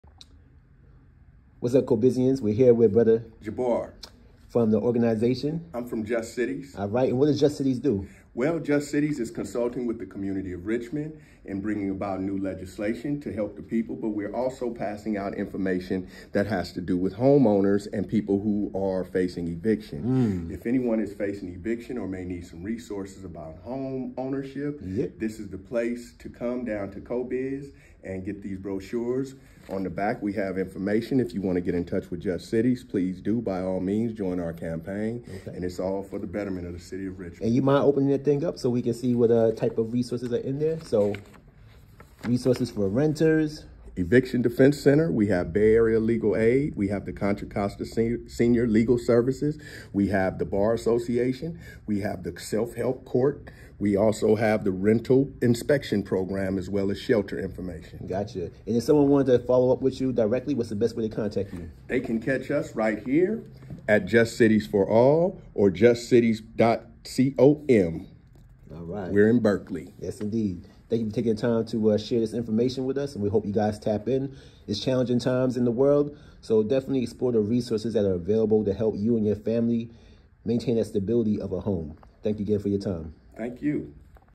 Community Resources: Facing Eviction? Jabar from Just Cities Institute
Much gratitude to Jabar Watson from Just Cities Institute for taking time out of his busy day to share resources to help homeowners and renters who are potentially facing eviction. It takes a village to build and sustain wellness which is why we welcome the builders to build with us.